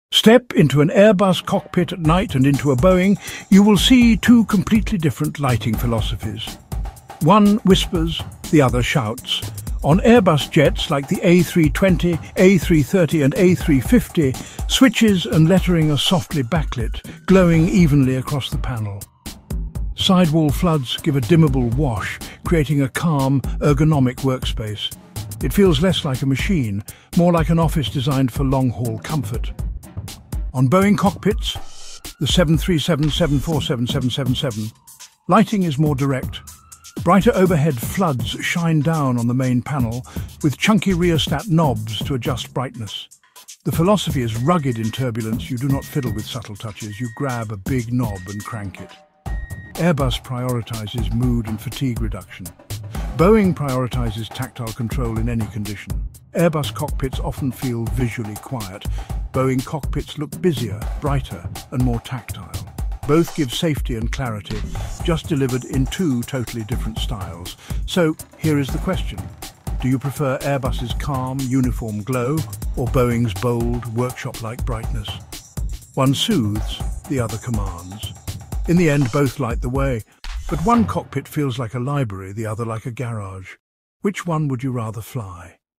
Airbus vs Boeing: Night Cockpit Secrets
Airbus vs Boeing cockpit lighting — calm glow vs bright command. 🌙 One soothes, the other shouts. Which flight deck would you rather face at 3am?